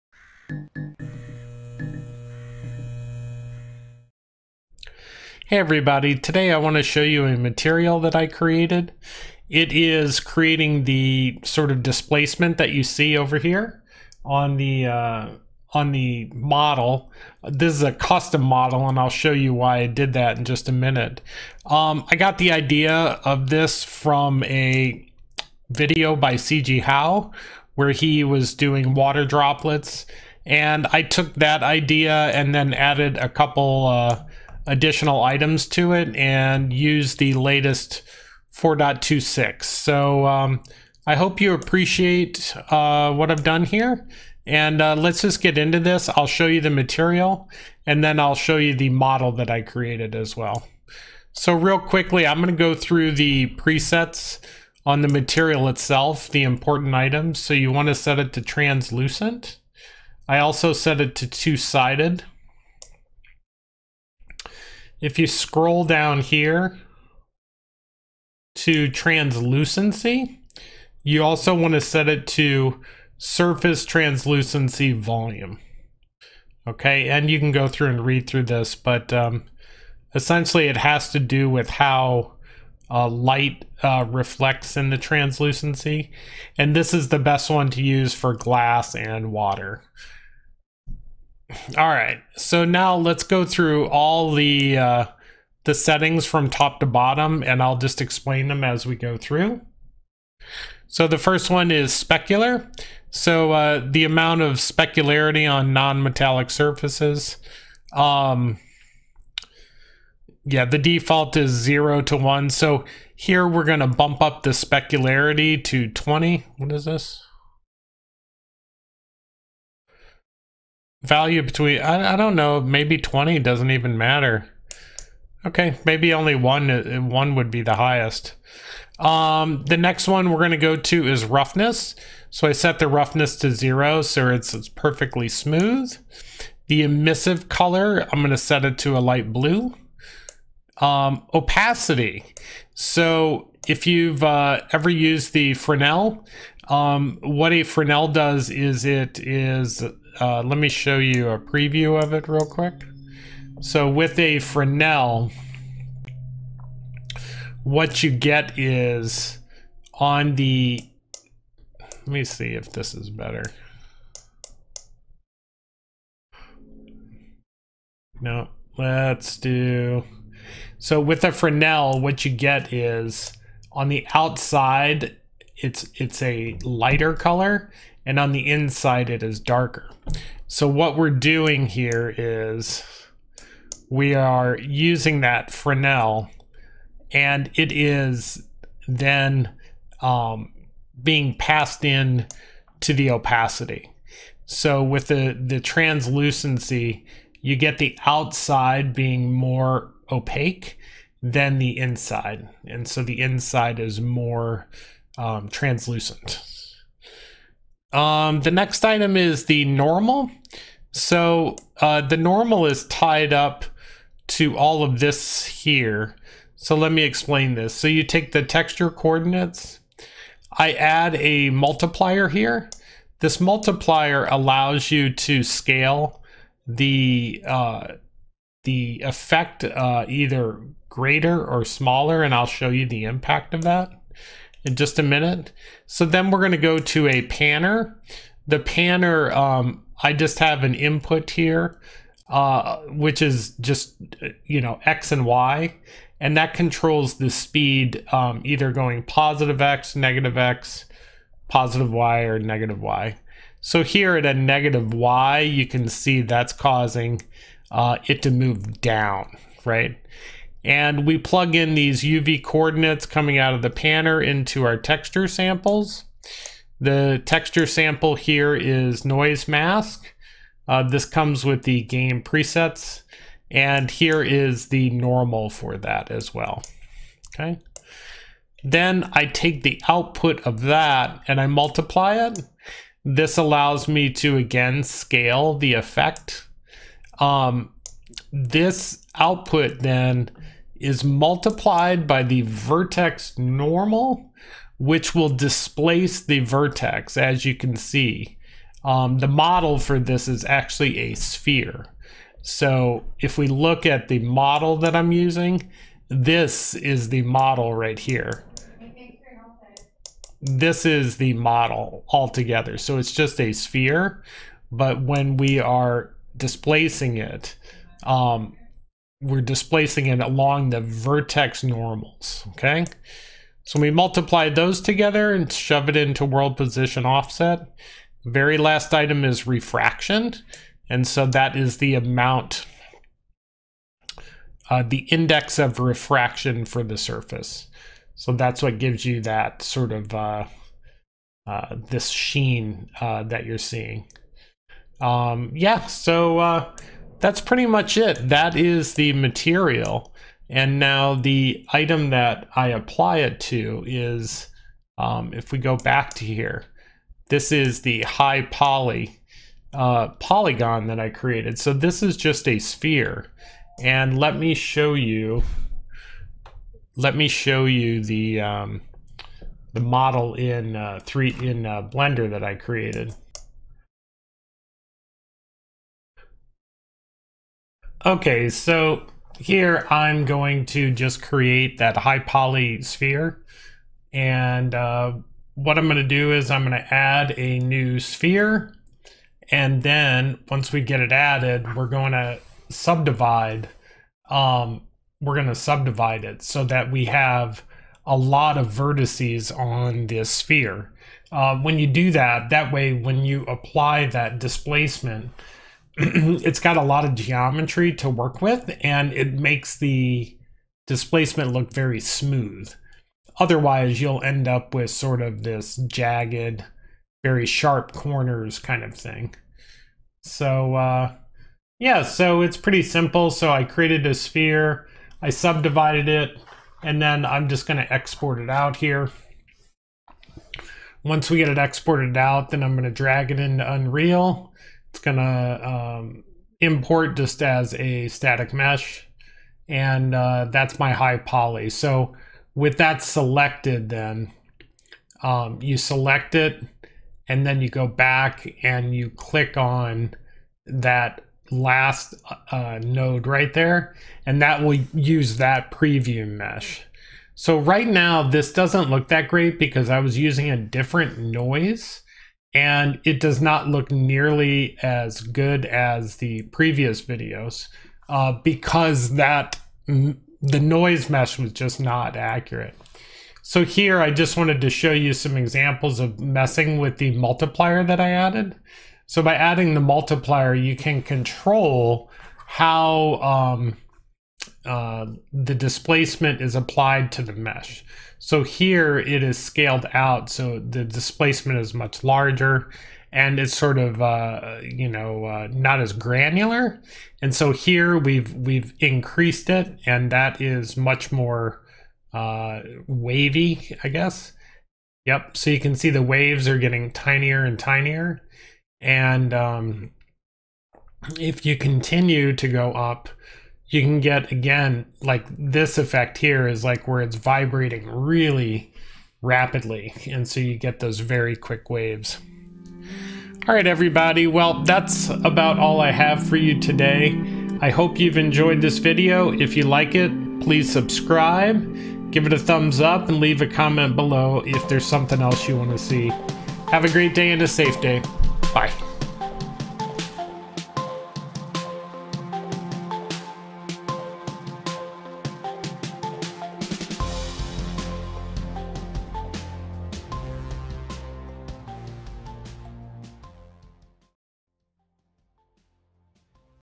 World Displacement Material (water)
In this video I use ue4 to create a material which leverages world displacement.
I also create a high polygon / vertex sphere that I used to preview the material.